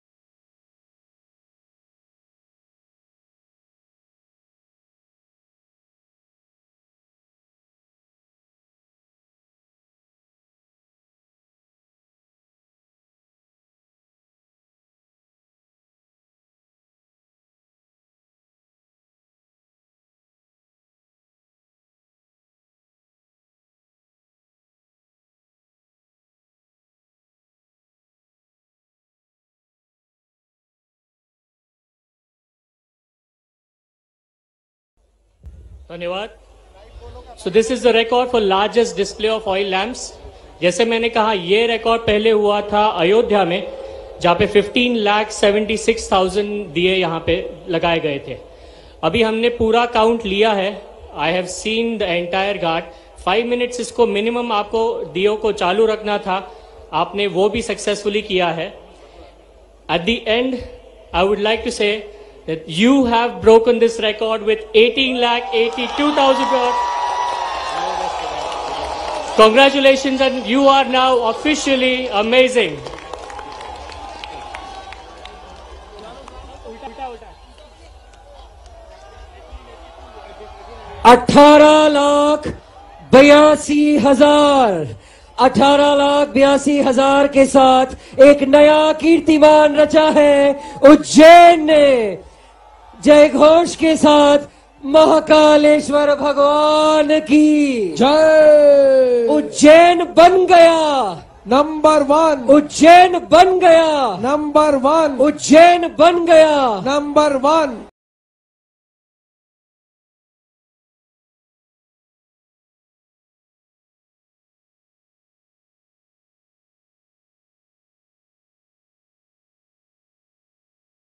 Ujjain में 18 लाख 82 हजार दीप प्रज्वलन का नया रिकॉर्ड
शिवरात्रि पर उज्जैन के महाकाल मंदिर में जहां एक तरफ बाबा भोले के दर्शनों के लिए श्रद्धालुओं की भीड़ उमड़ी, वहीं दूसरी ओर क्षिप्रा नदी के तट पर एक साथ 18 लाख 82 हजार दीये जलाने का वर्ल्ड रिकॉर्ड बना। इससे पूर्व अयोध्या के दीपोत्सव में एक साथ 15 लाख 76 हजार दीये जलाने का रिकॉर्ड बना था।